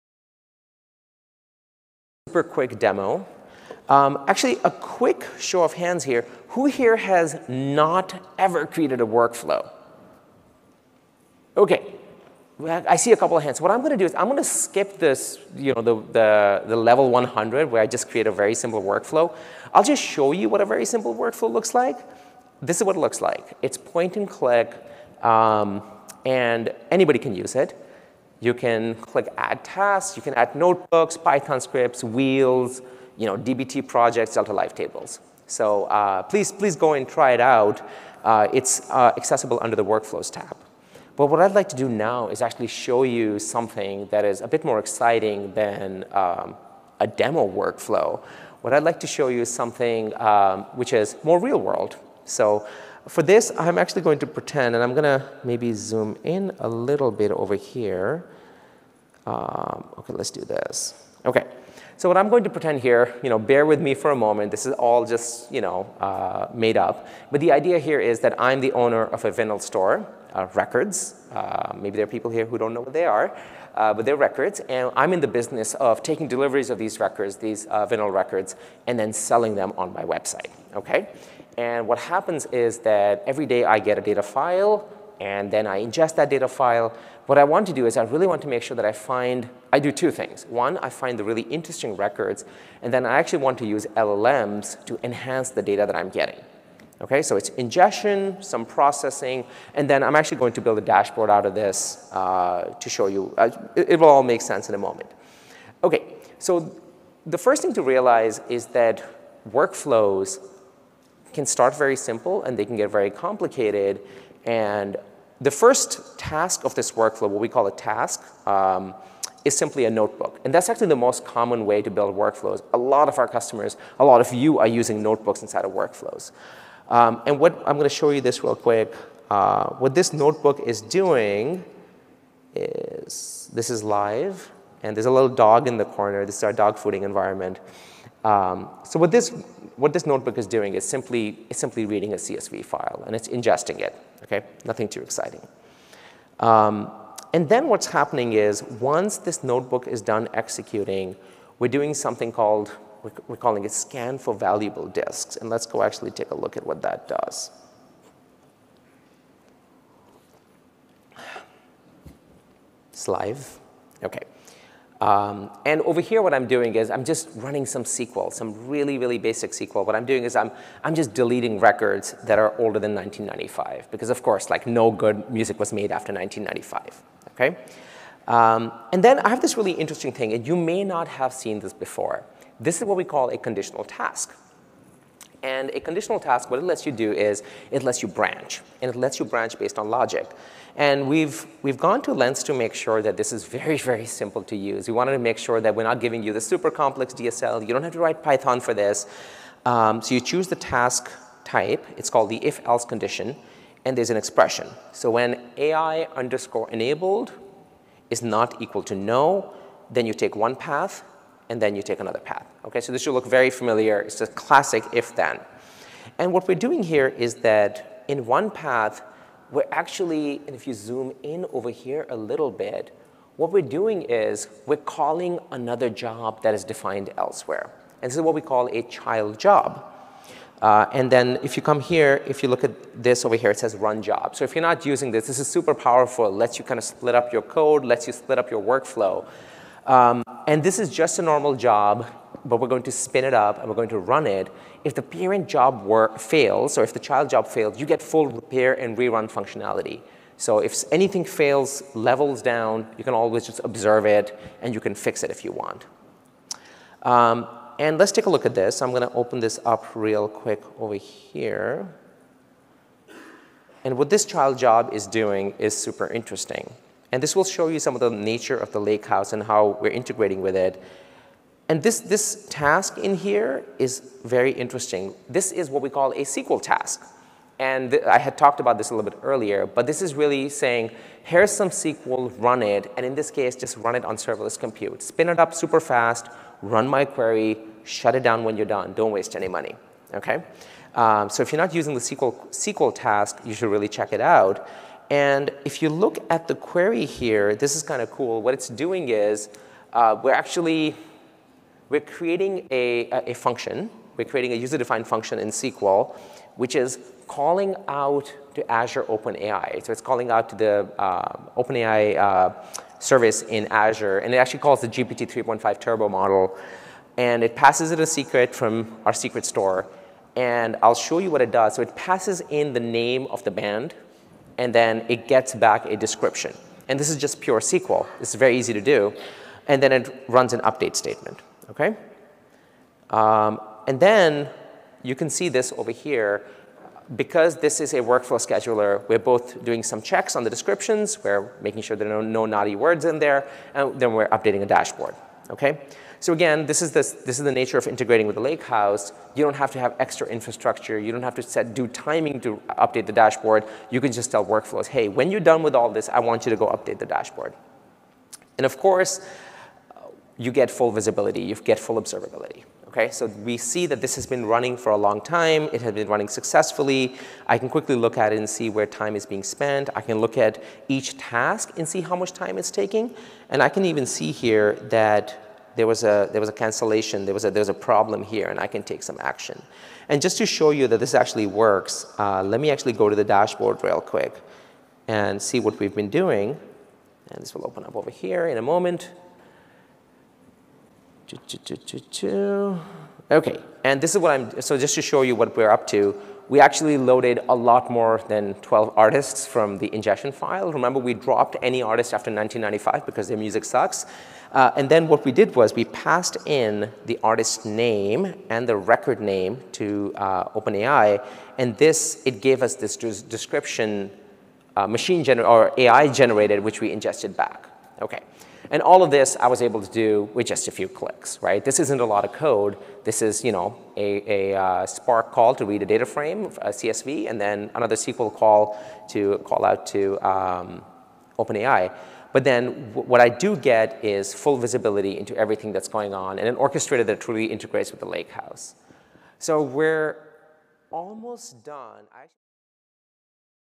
What's new in Databricks Workflows
DAIS 2023 demo about new features in Databricks Workflows

Note: Databricks Lakeflow unifies Data Engineering with Lakeflow Connect, Lakeflow Spark Declarative Pipelines (previously known as DLT), and Lakeflow Jobs (previously known as Workflows).